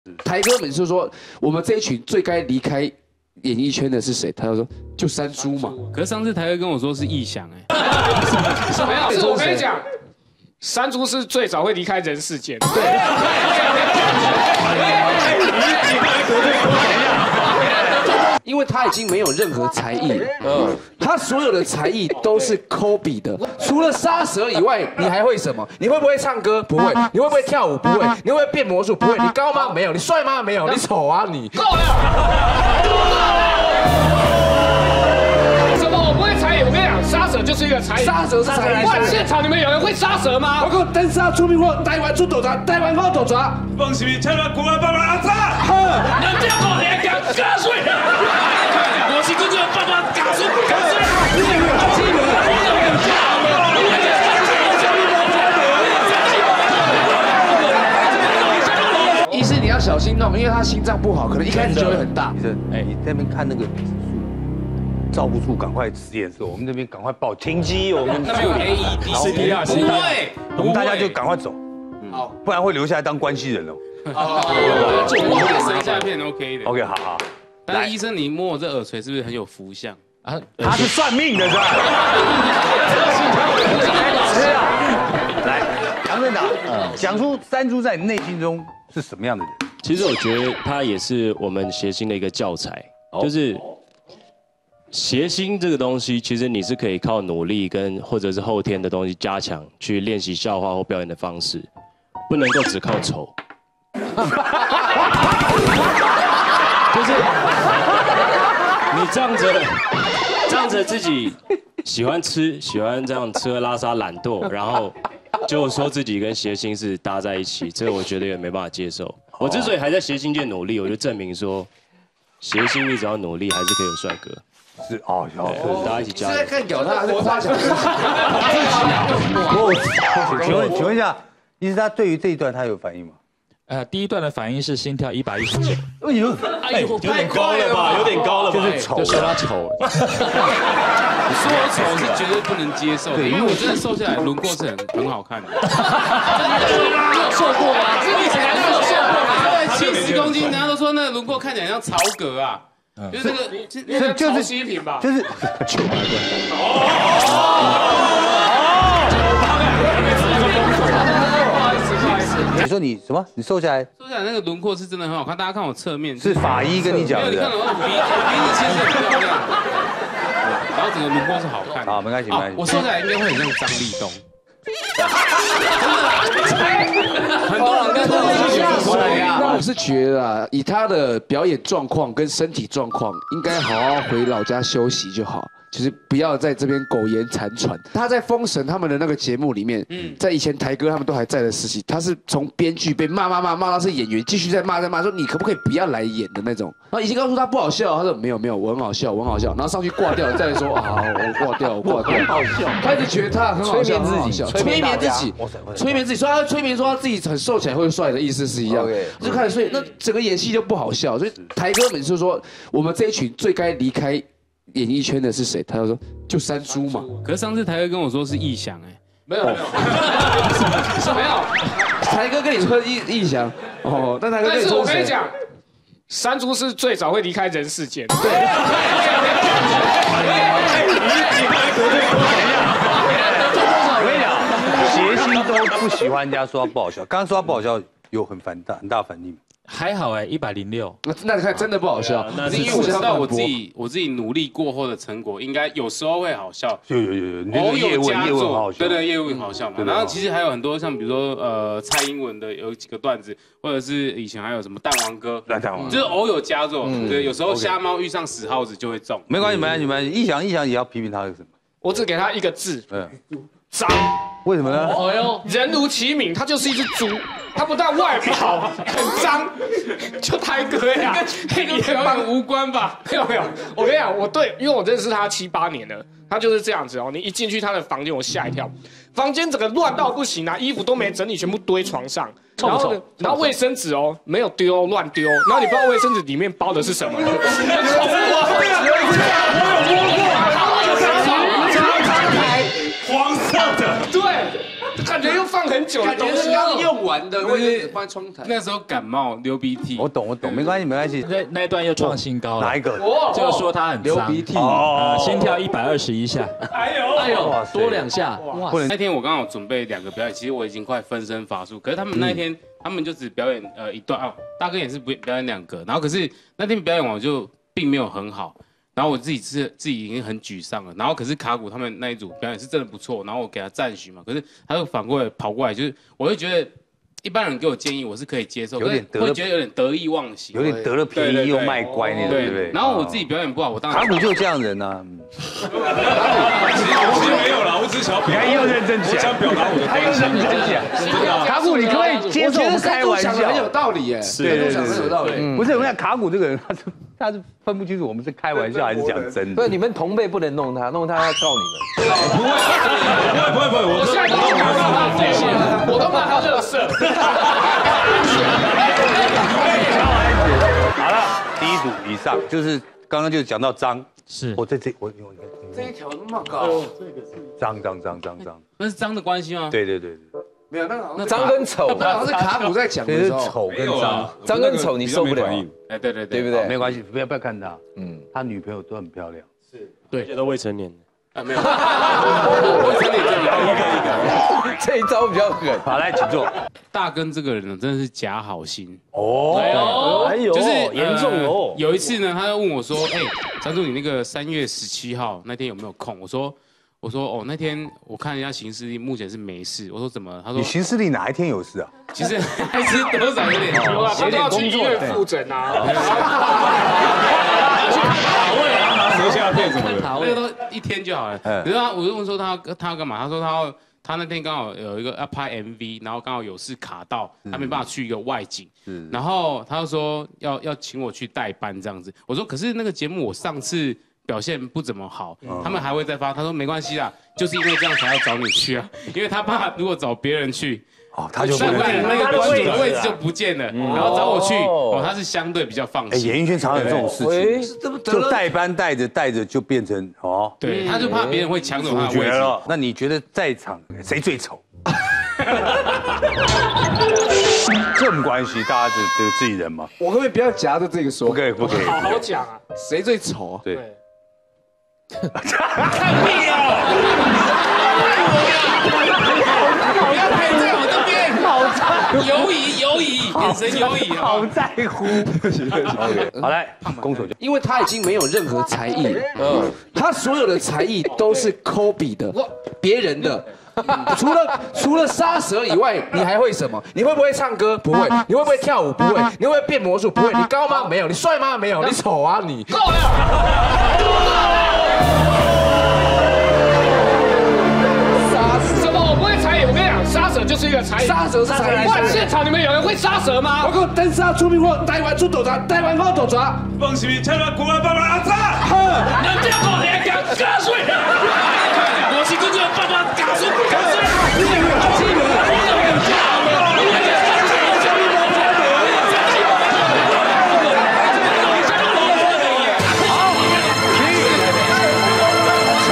山豬 好感情一起表演拿手絕活 友人神預言::最早會離開人世間 R.I.P │ Vidol
諧星 湊在一塊兒表演往往代表你/妳即將擁有一段快樂的時光😇